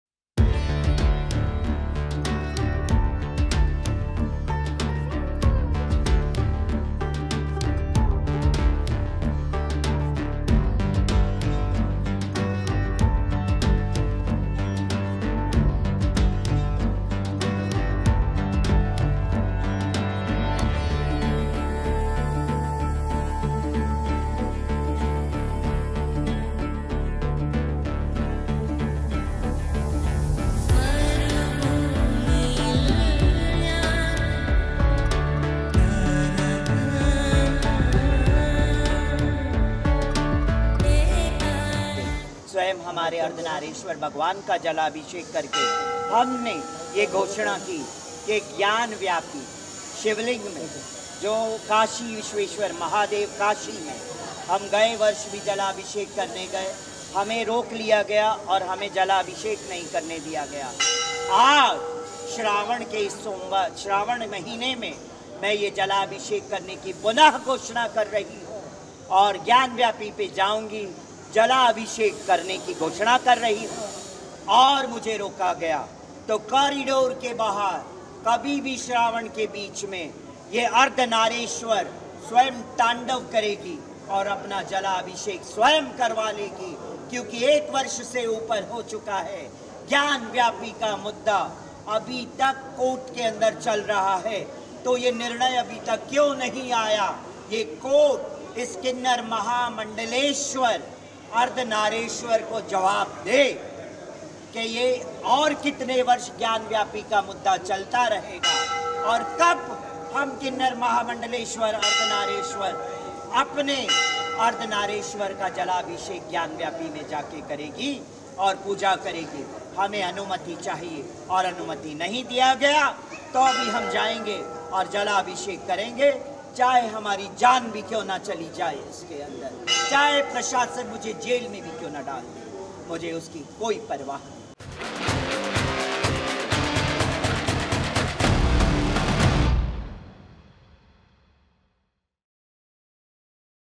Gyanvapi को लेकर किन्नर महामंडलेश्वर हिमांगी की खुली चुनौती !
Gyanvapi Case:  पहली किन्नर महामंडलेश्वर हिमांगी ने एलान किया है कि Gyanvapi में जलाभिषेक करेंगी और उन्हें इससे कोई नहीं रोक सकता.